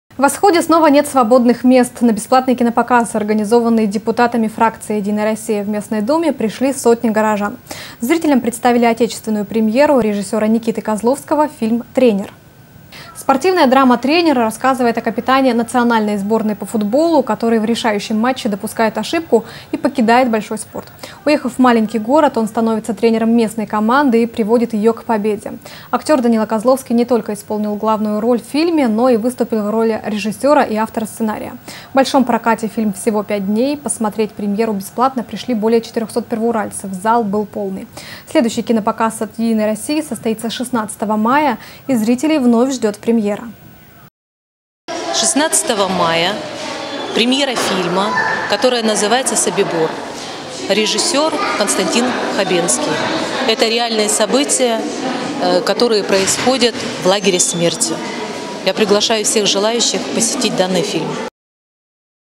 В "Восходе" снова аншлаг
На бесплатный кинопоказ, организованный депутатами фракции "Единая Россия" в местной Думе, пришли сотни горожан. Зрителям представили отечественную премьеру  режиссера Никиты Козловского -  фильм "Тренер".

Спортивная драма "Тренер" рассказывает о капитане национальной сборной по футболу, который в решающем матче допускает ошибку и покидает большой спорт. Уехав в маленький город, он становится тренером местной  команды и приводит ее к победе. Актер Данила Козловский не только исполнил главную роль в фильме, но и выступил в роли режиссера и автора сценария. В большом прокате фильм всего 5 дней. Посмотреть премьеру бесплатно пришли больше 400-т первоуральцев - зал был полный. Следующий кинопоказ от "Единой России" состоится  16 мая. И зрителей вновь ждет премьера.

Наталья ВОРОБЬЕВА, секретарь местного отделения партии "Единая Россия": «16 мая - премьера фильма, который называется "Собибор". Режиссер - Константин Хабенский. Это реальные события, которые происходят в лагере смерти. Я приглашаю всех желающих посетить данный фильм».